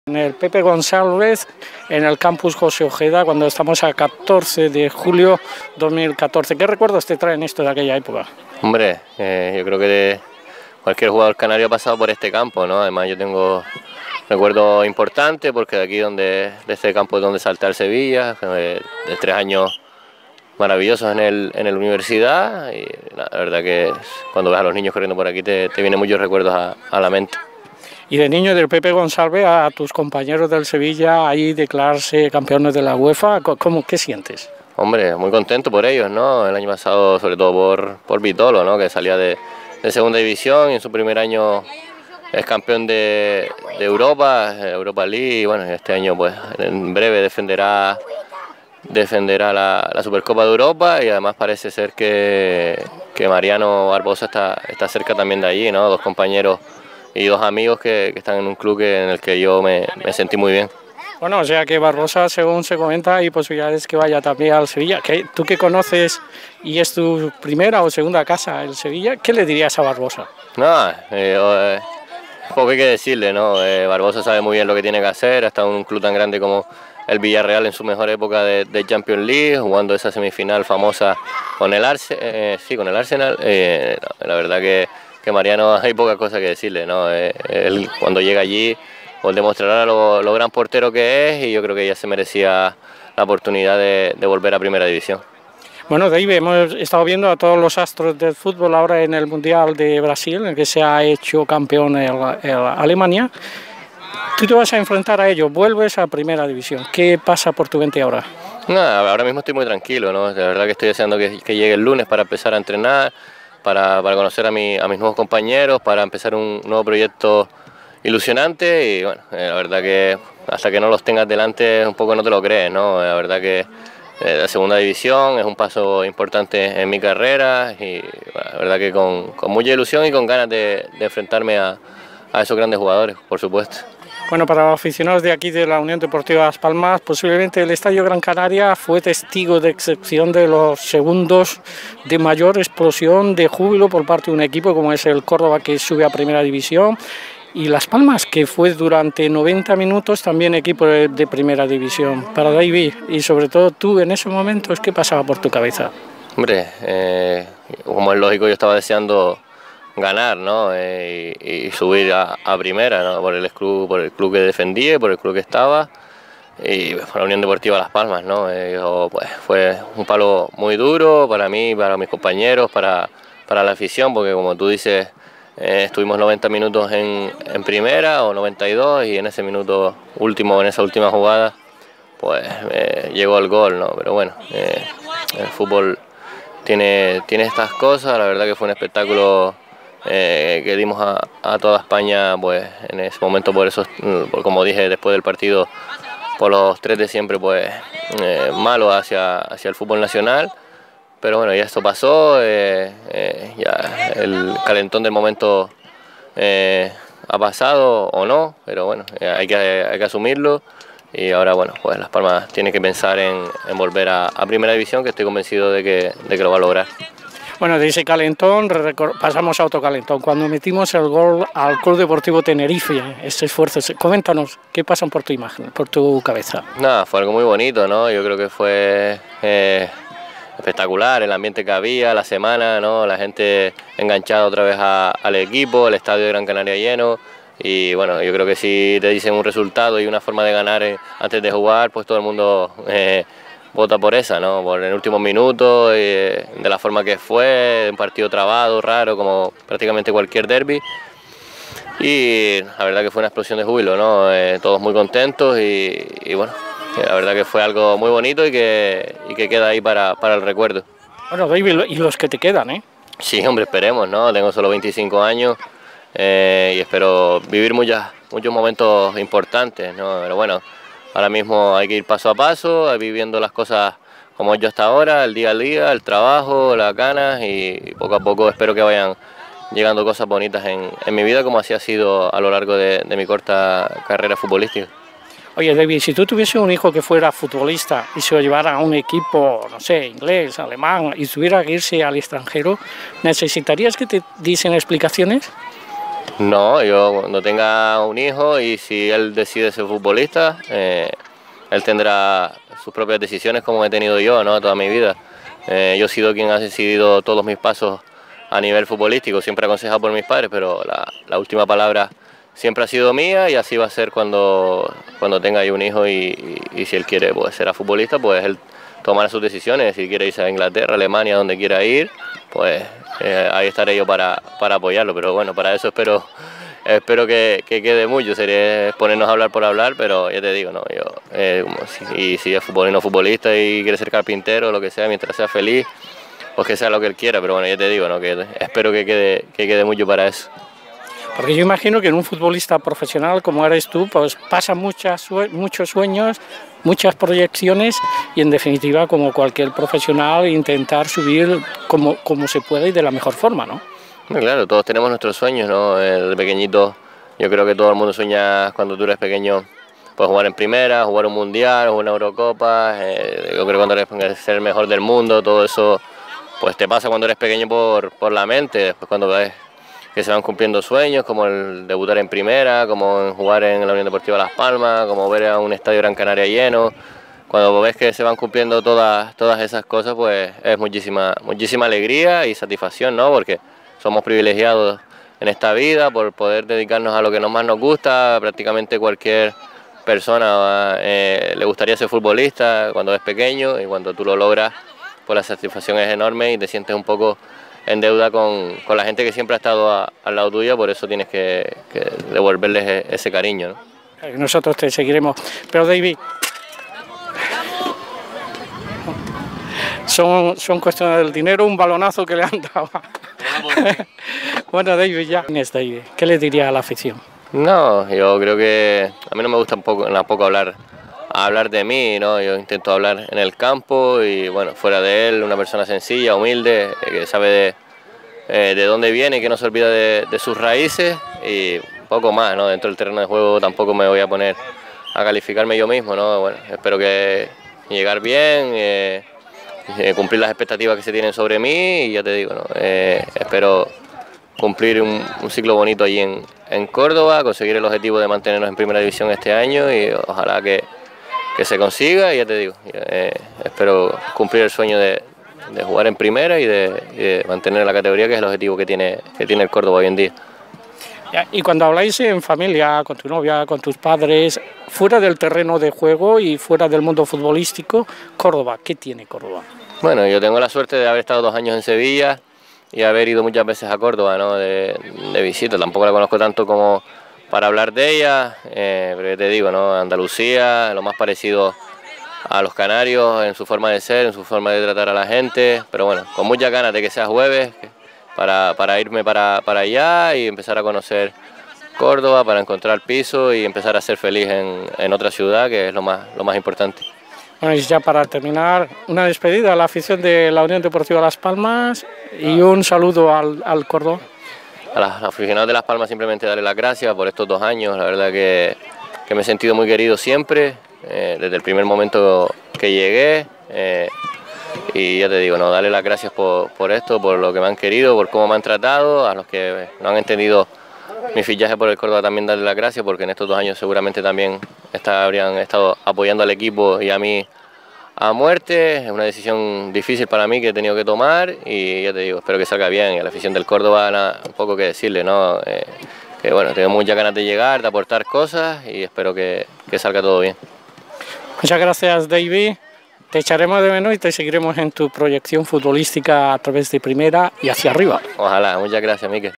Deivid en el Campus de verano Jose Ojeda.
Pepe Gonçavez en las Palmas de Gran Canaria 2014.
Mykel /canarPress
El futbolista Deivi, regresa a la primera división con el Cordoba CF.
Tras sus pasos por el Sevilla CF, UD Las Palmas, Universidad CF, Union Viera y Junta de puerto. Pero antes compartía unas horas con los participantes del campus.

P.D.: La entrevista se realiza a pie de campo justo en el banquillo Local cuando el en el minuto 9:09 la camara recibe un balonaza de un chaval. No paso nada y so cosas del directo de ese momento...